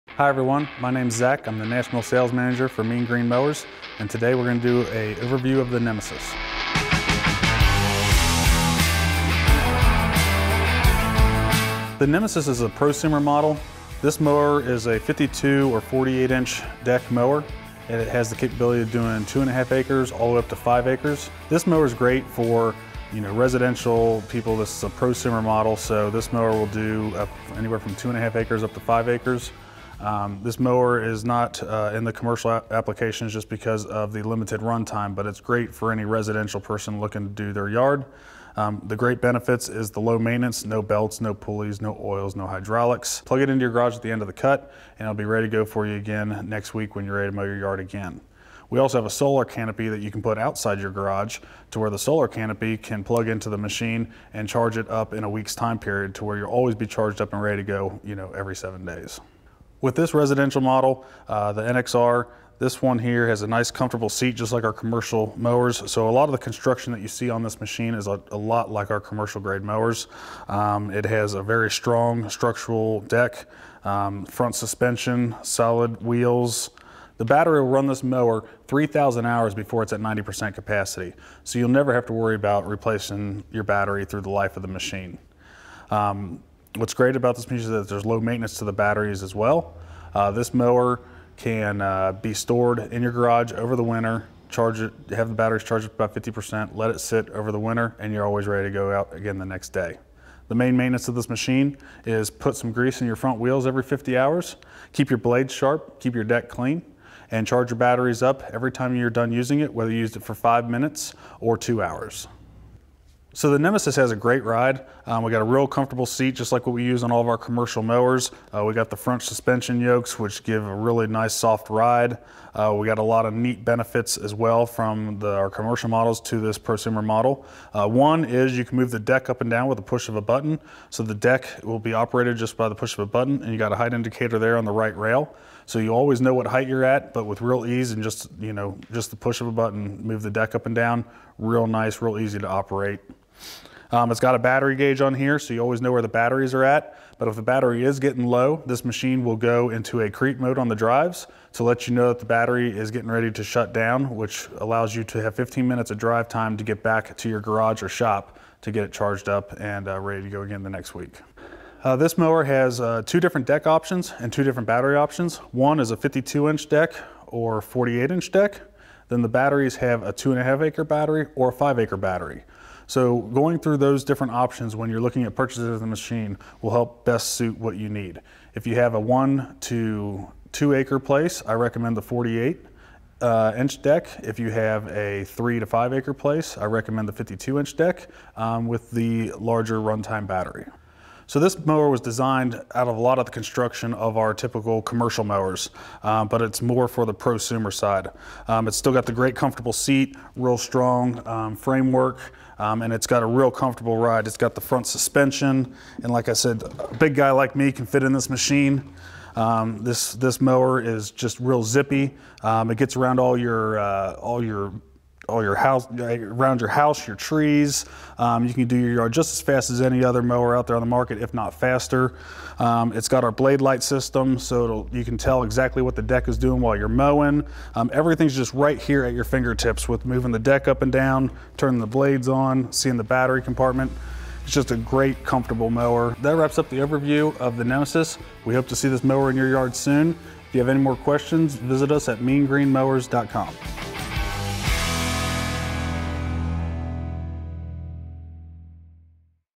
NEMESIS NXR-48/52
When you combine the brute power of Mean Green’s CXR and the stealth of Mean Green’s Stalker, you have the next step in Mean Green Mowers’ evolution, the Nemesis NXR. Crafted out of lightweight aluminum and high strength steel, the Nemesis is strong and agile. The NXR is designed with a low center of gravity and optional Rite-Ride suspension. With zero emissions, zero gas, low noise and little maintenance, the Nemesis is bringing the Electric advantage to any home or job.